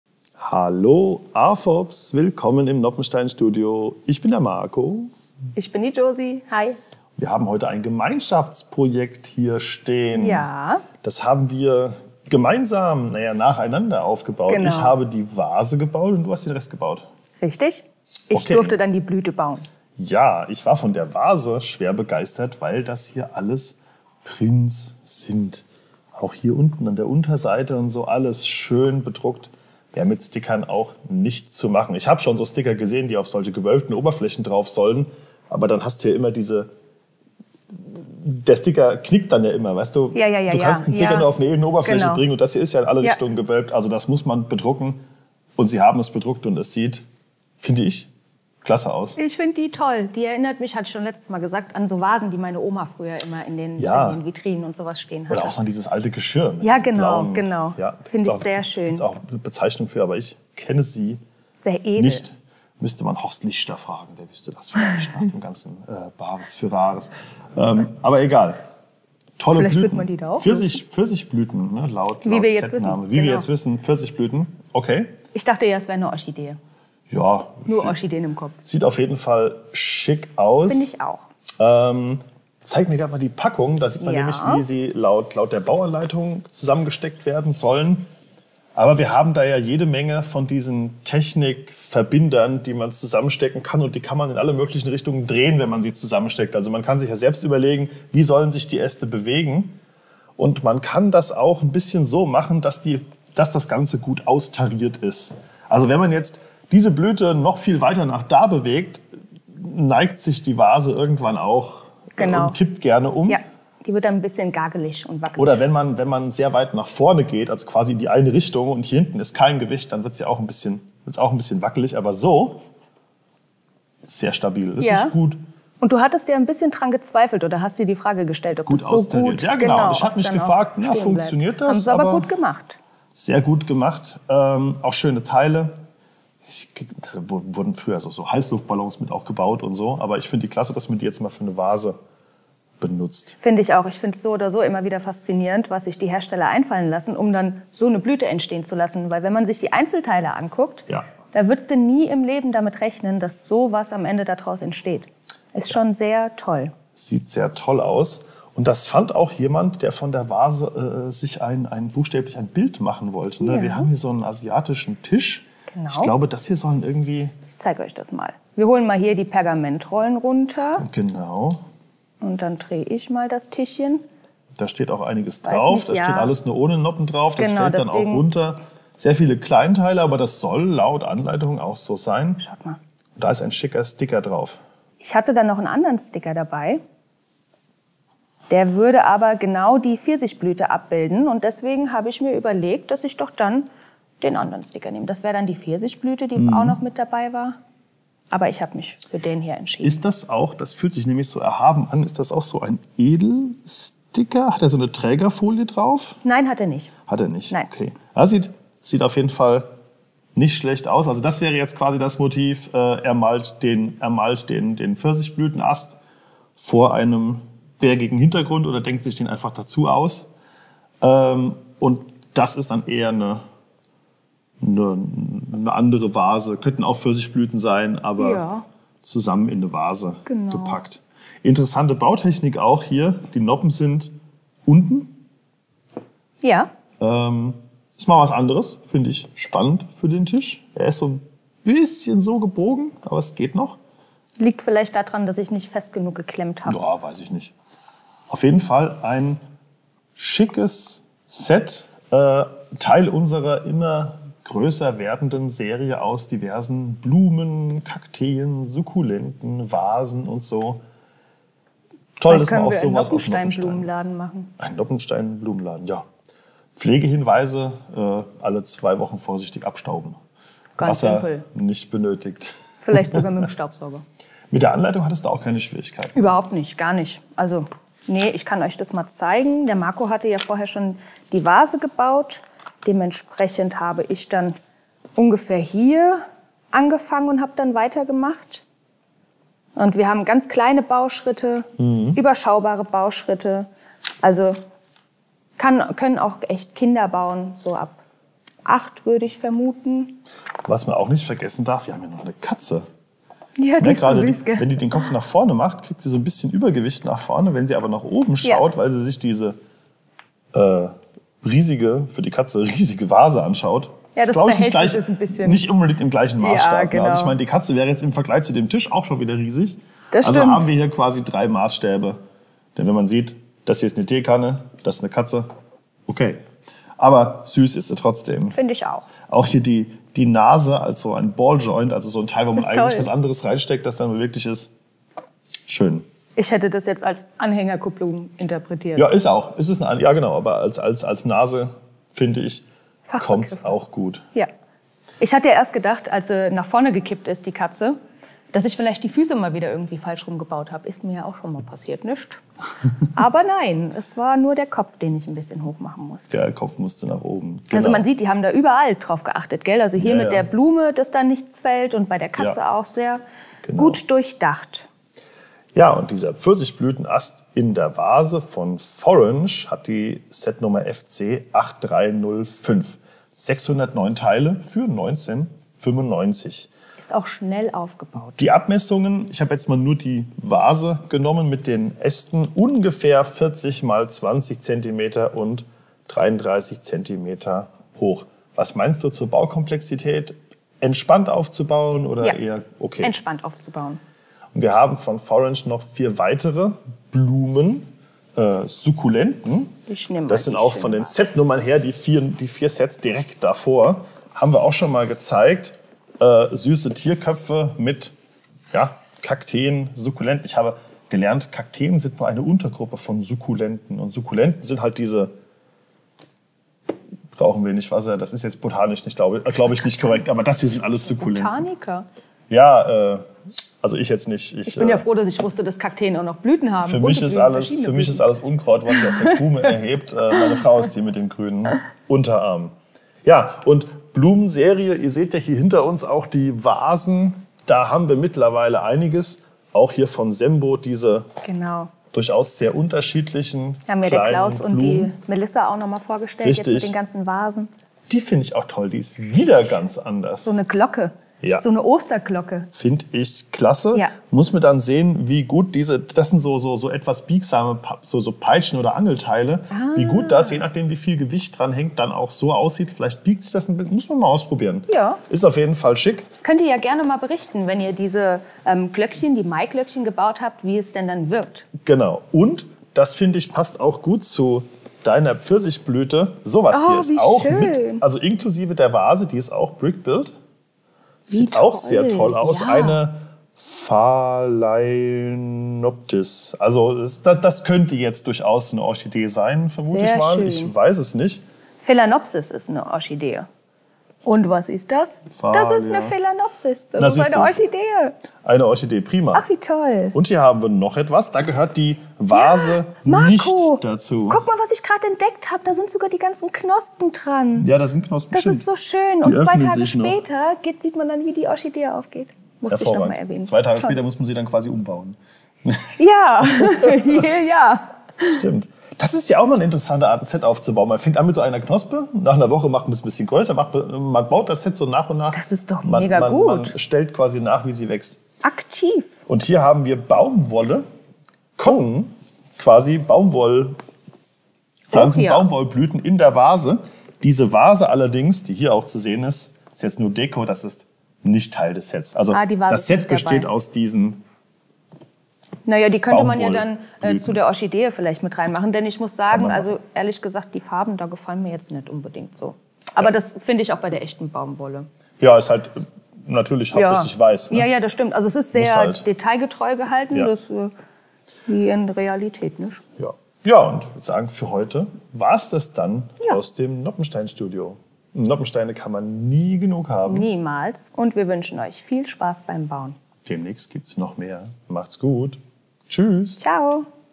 Pfirsichblütenast in einer Vase von Forange @ BlueBrixx
Heute stellen Josie und Marco ein Gemeinschaftsprojekt vor: Die herrlich bedruckte Vase des Pfirsichblütenasts hat Marco aufgebaut, den Rest des Sets hat Josie zusammengesteckt. Das Modell von Forange zeigt den namensgebenden Ast, eine süße Katze und einen Tisch, auf dem ein Bild der Pfirsichblüten angefertigt wird.

Das Set aus 609 Teilen passt hervorragend zu unseren anderen Blumen und Vasen. Ab 6:34 zeigt Marco einige dieser Sets, darunter auch eine edle Phalaenopsis-Orchidee in einer Vase.

06:46 Neue Blumen-Serie von Forange @ BlueBrixx - diese Sukkulenten brauchen kein Wasser!
07:26 Blumen mit Vase von LiMei @ BlueBrixx - und jede Menge Sembo-Blumen!